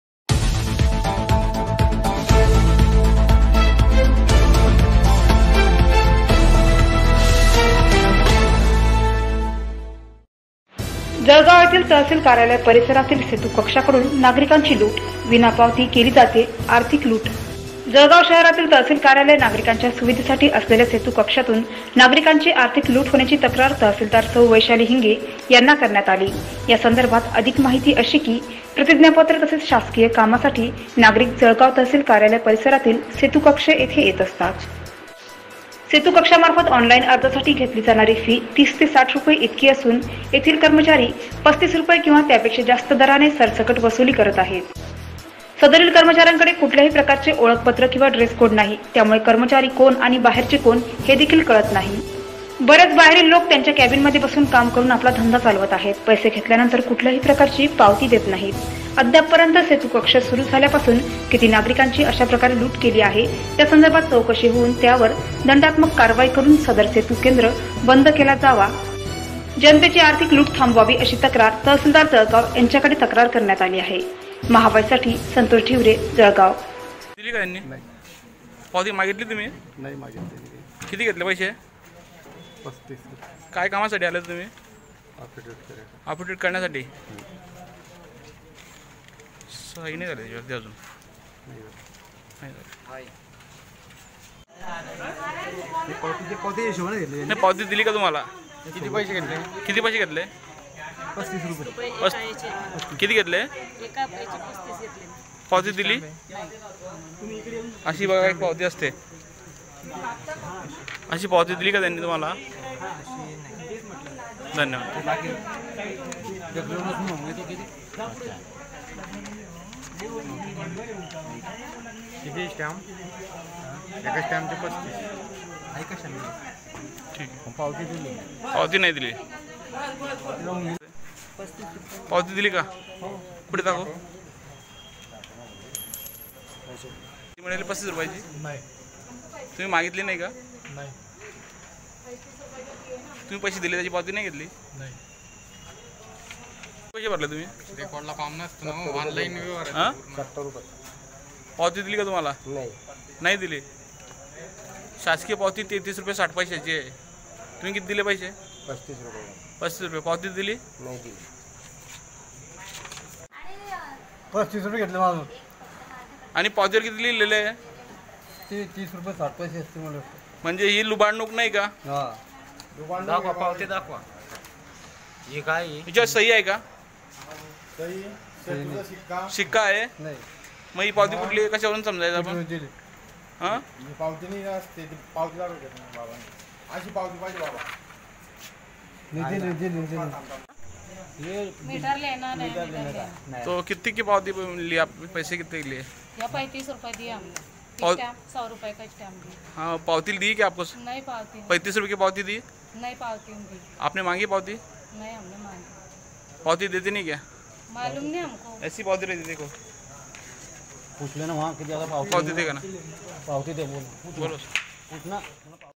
जळगाव - तहसील कार्यालय परिसरातील सेतू कक्षाकडून नागरिकांची लूट...
जळगाव शहरातील तहसील कार्यालय नागरिकांच्या सुविधेसाठी असलेल्या सेतु कक्षातून नागरिकांची आर्थिक लूट होण्याची तक्रार तहसीलदार सौ वैशाली हिंगे यांना  करण्यात आली या संदर्भात अधिक माहिती अशी की  प्रतिज्ञापत्र तसेच शासकीय कामासाठी नागरिक जळगाव तहसील कार्यालय परिसरातील सेतू कक्ष येथे येत  असतात . सेतु कक्षामार्फत ऑनलाइन अर्जासाठी घेतली जाणारी फी ते 30 रुपये 60 पैसे इतकी असून तेथील कर्मचारी 35 रुपये किंवा त्यापेक्षा जास्त दराने सरसकट वसुली करत आहेत . सदरील कर्मचाऱ्यांकडे  कुठलेही प्रकारचे  ओळखपत्र  किंवा  ड्रेस कोड  नाही . त्यामुळे  कर्मचारी  कोण आणि बाहेरचे  कोण  हे देखील  कळत नाही. बरेचसे बाहेरील  लोक त्यांच्या केबिनमध्ये  बसून काम करून  आपला  धंदा  चालवत असतात .पैसे घेतल्यानंतर कुठल्याही प्रकारची पावती देत नाहीत. अद्याप पर्यंत सेतू कक्ष सुरू झाल्यापासून किती नागरिकांची अशाप्रकारे लूट केली आहे त्या संदर्भात चौकशी होऊन त्यावर दंडात्मक कारवाई करून सदर सेतू केंद्र बंद केला जावा . जनतेचि आर्थिक लूट थांबवावी अशी तक्रार  तहसीलदार जळगाव यांचेकडे तक्रार करण्यात आली आहे. महा वाईस टीव्ही चॅनेल साठी संतोष ढिवरे जळगाव